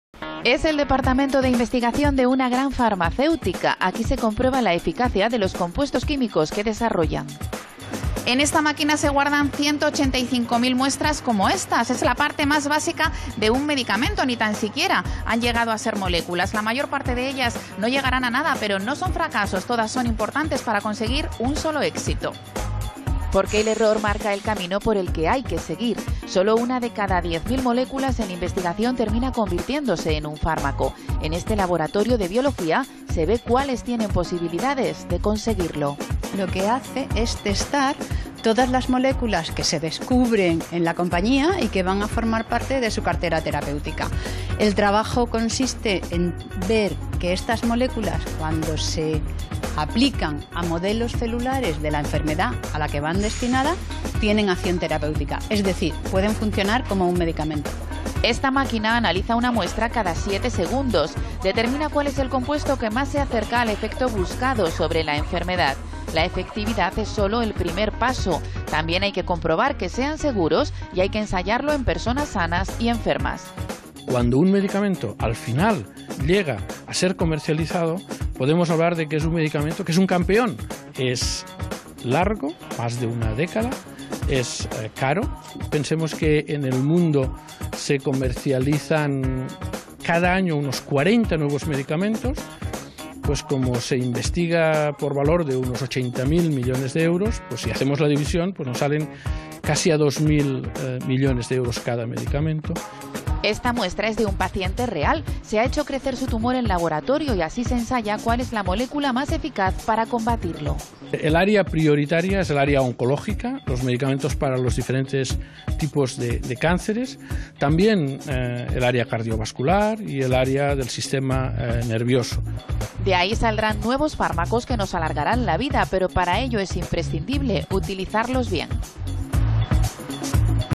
La historia de los medicamentos 'campeones'
Reportaje de TVE sobre cómo se lleva a cabo la investigación y desarrollo de los nuevos medicamentos. El reportaje muestra cómo funciona una planta de I+D de una gran compañía farmacéutica española e incluye una entrevista con el director técnico de Farmaindustria, Emili Esteve, quien explica todo el proceso y defiende que los medicamentos que llegan finalmente al paciente tras un proceso de muchos de ensayos son auténticos "campeones".